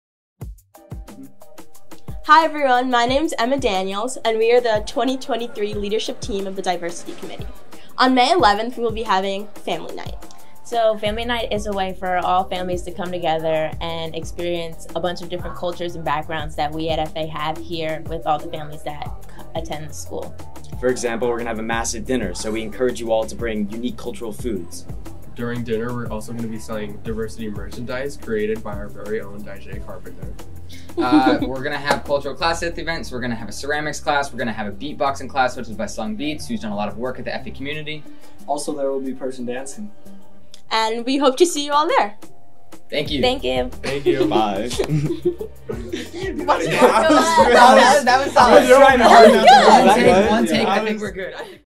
Family Night at Friends Academy
On Wednesday, May 11, bring your favorite family recipe, pick an activity to do with friends, and share in an evening that explores and honors the beautifully diverse cultures among us.